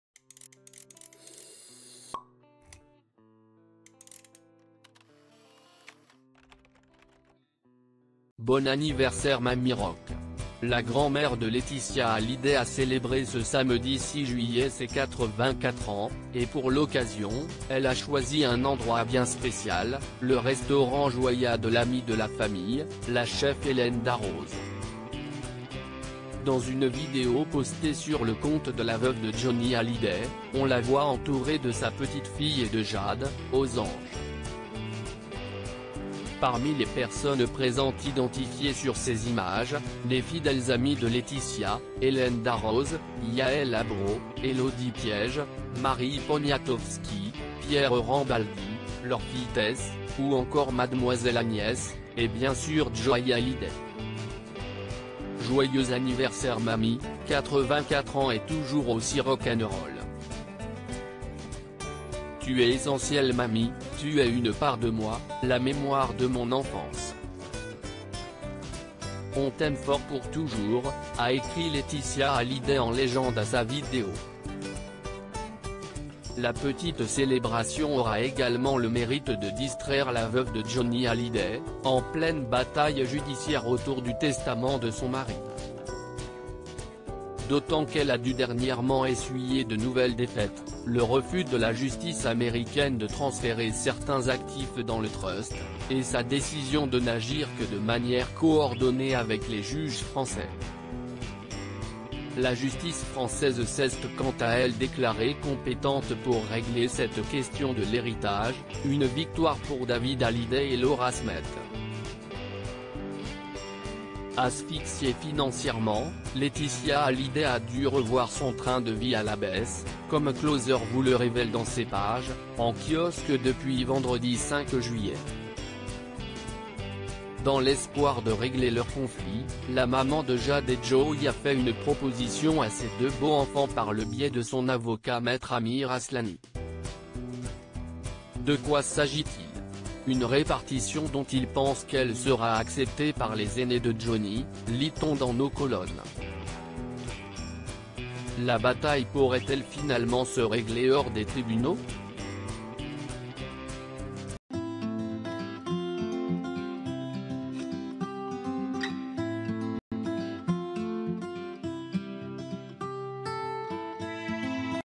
Mamie Rock fête son anniversaire entourée de Laeticia et Jade Hallyday au restaurant d'Hélène
Mamie Rock fête son anniversaire entourée de Laeticia et Jade Hallyday au restaurant d'Hélène Darroze